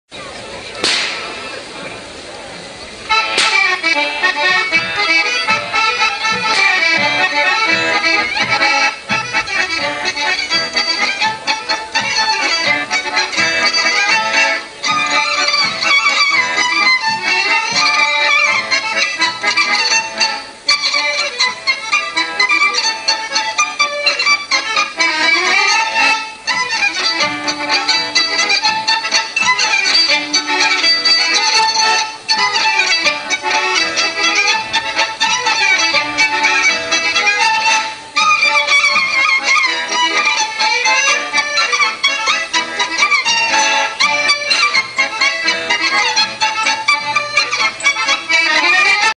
Oberek po suwalsku
Tradycyjny lokalny oberek grany przez muzykantów – członków kapeli „Przeroślaki”: Konstantego Olszewskiego- skrzypce i  Zygmunta Ułanowicza (harmonia trzyrzędowa).
Nagranie z Węgorzewskiego Jarmarku Folkloru 1987 r. Zdigitalizowane przez Suwalski Ośrodek Kultury w ramach projektu MKiDN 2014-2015.